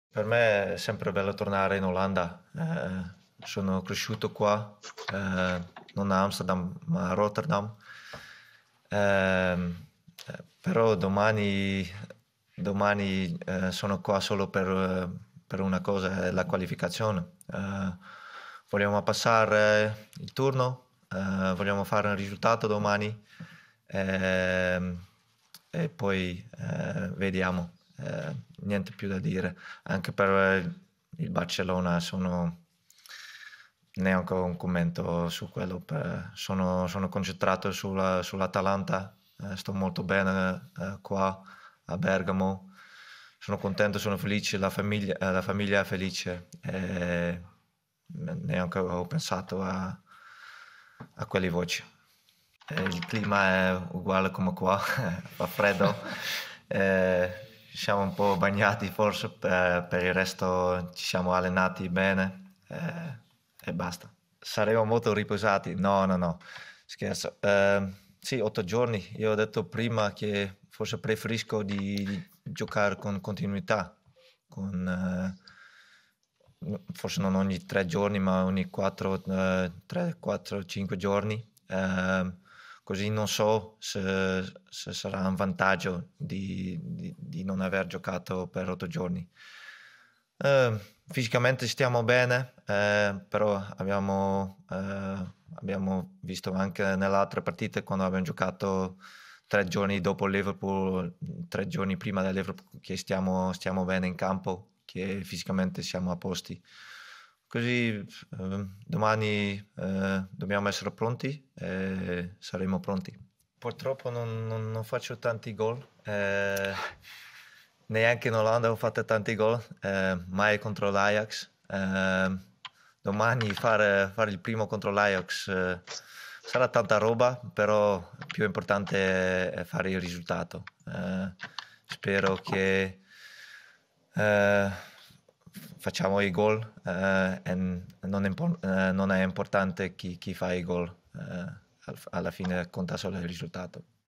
Ajax-Atalanta, De Roon: "Siamo qui per passare il turno"
"Stiamo bene fisicamente, fare gol all'Ajax sarebbe tanta roba"

SPORT (Amsterdam). "È sempre bello tornare in Olanda, anche se sono cresciuto a Rotterdam. Sono qui per una sola cosa, passare il turno. Poi vedremo tutto il resto. Il Barcellona? Non ho nessun commento da fare, sono concentrato sull'Atalanta. Sto bene qui, la famiglia è felice. Non ho pensato alle voci di mercato". Così Marten De Roon centrocampista dell'Atalanta alla vigilia della sfida di Amsterdam contro l'Ajax. "Purtroppo non faccio tanti gol, non ho mai segnato contro l'Ajax. Sarebbe tanta roba farlo domani, ma l'importante rimane il risultato. Non sarà importante chi segnerà, ma solo il risultato". (Antonio Lopopolo/alanews)